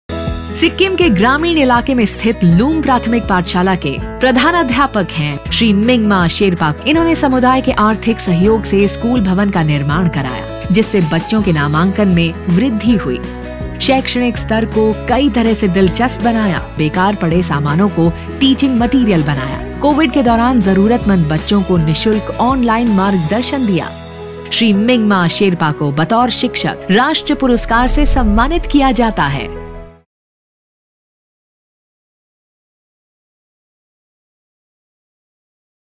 Shri Mingma Sherpa, Head Teacher, Sikkim
Shri Mingma Sherpa has done remarkable work to raise funds from the community to improve his school’s infrastructure, increase the enrollment of students and retain them in school by improving academic standards and the quality of the mid-day meal.